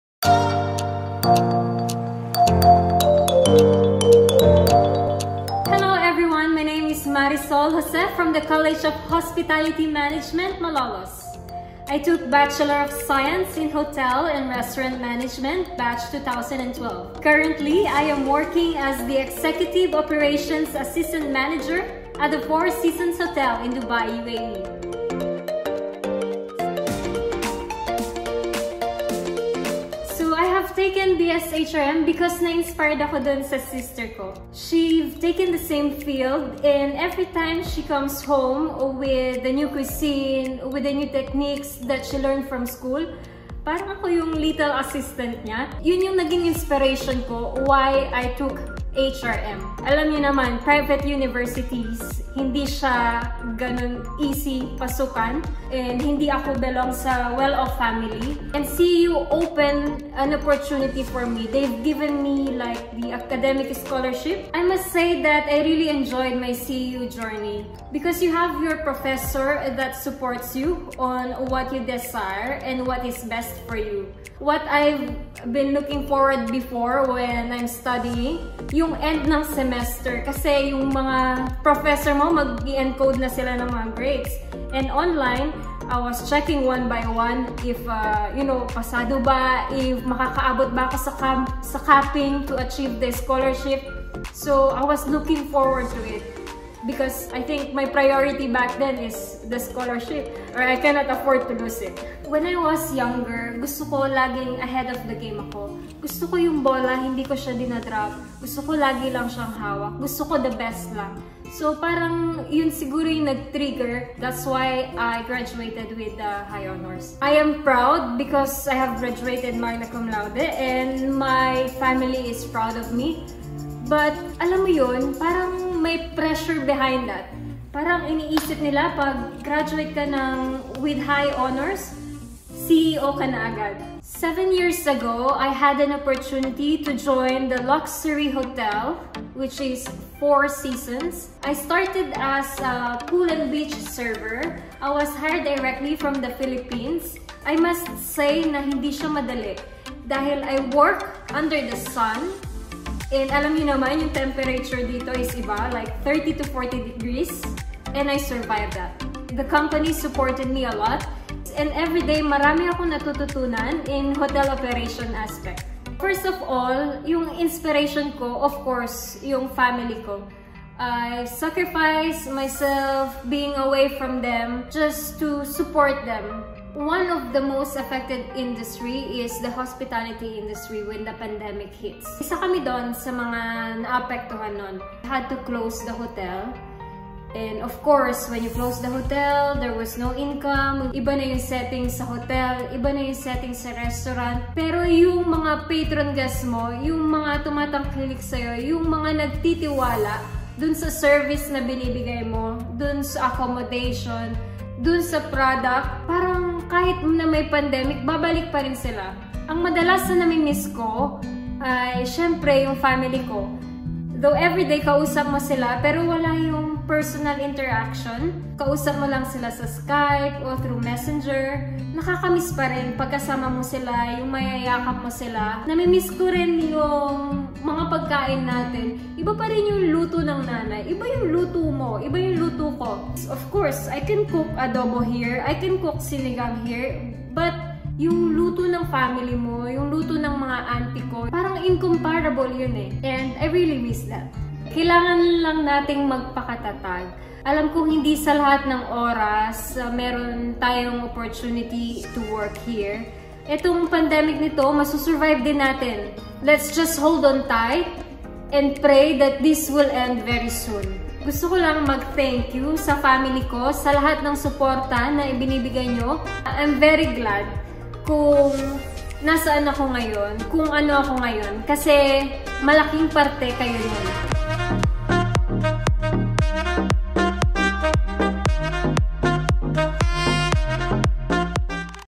CEUFridayFeatures presents Escolarian Executive Operations Manager in Dubai, UAE | Marisol Jose
Get to know the Escolarian who became successful in the international hospitality scene.

Watch our CEUFridayFeatures interview with Ms. Marisol Jose and be inspired with the story of how her simple dreams turned to great achievements. 💜